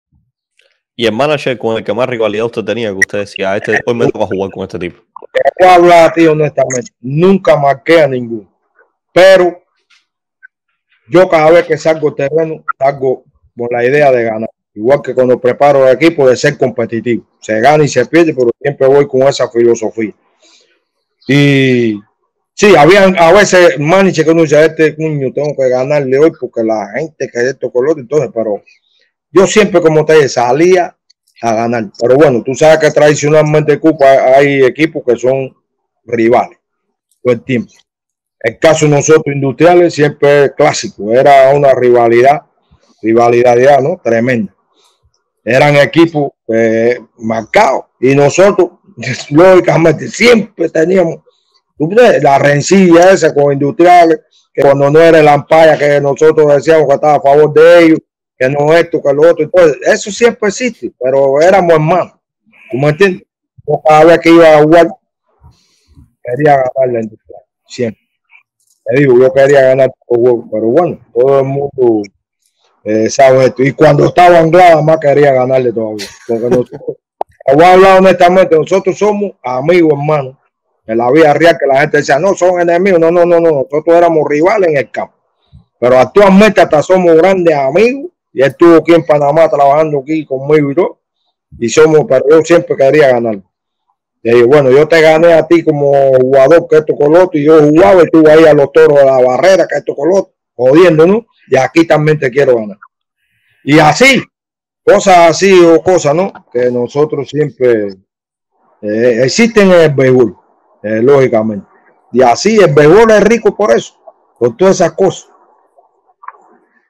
Alfonso Urquiola habla de su rivalidad con Rey Vicente Anglada 🔥
El histórico Manager de Series Nacionales Alfonso Urquiola habla de su rivalidad con Rey Vicente Anglada. Uno de los duelos más grandes del beisbol cubano, Industriales vs pinar del Río.